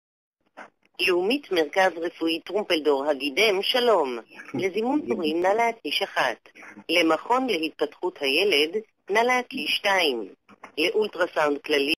Trumpeldor Hagidem at Leumit
Jerusalem street haGidem (the Amputee) was eventually renamed to Josef Trumpeldor, but the Leumit clinic branch on that street took a bit longer to get used to the new name.